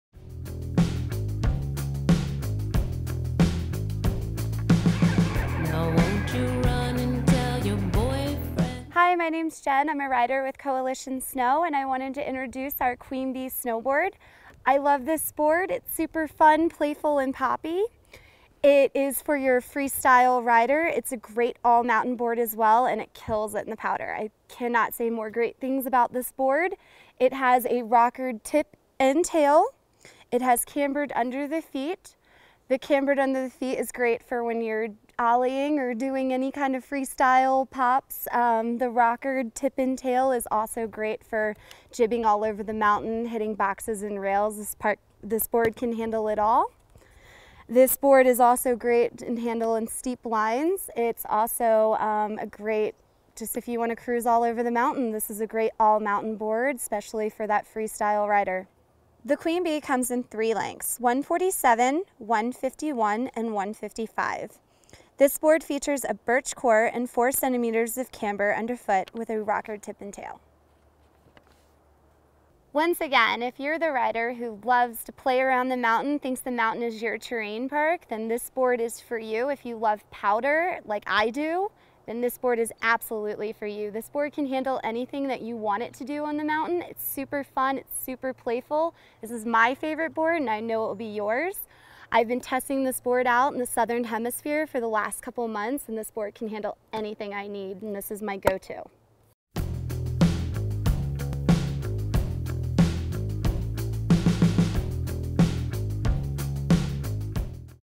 Queen Bee All Mountain Snowboard
You don’t have to tell us that you rule the mountain. Your style says it all.

ROYALTY If you love big mountain, pow shredding, fast groomers, and park laps all in the same day, the twin-tip Queen Bee is your board of choice. The rockered tip and tail creates a playful feel for jibbing and floating through powder, but the camber underfoot makes this board stable through all other kinds of terrain. Its softer flex allows for easy turning but don't get us wrong—this board can handle the steeps as well.